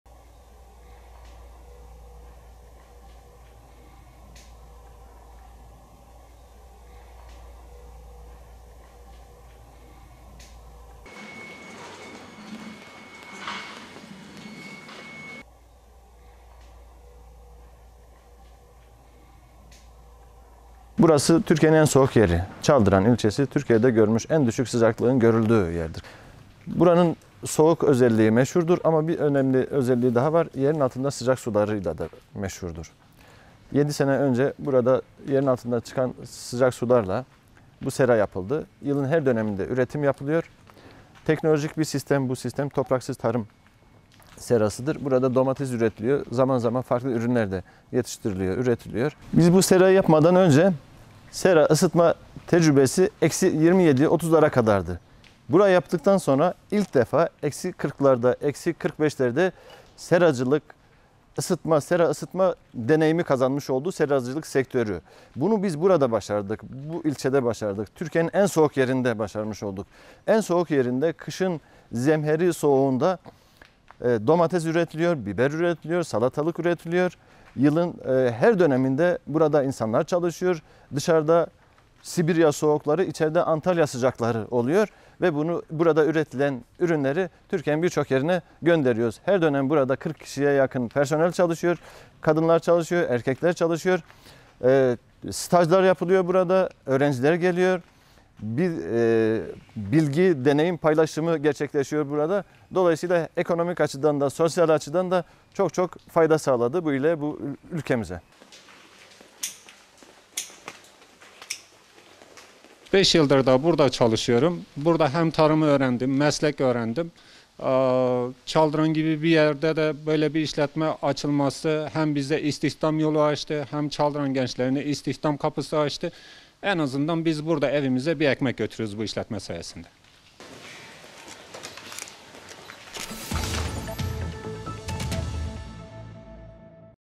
Dondurucu soğukta termalle 1500 ton domates üretildi
Kış mevsiminin sert geçtiği Van'ın Çaldıran ilçesinde, jeotermal enerji ile ısıtılan serada yetiştirilen domatesler ülkenin birçok bölgesine gönderiliyor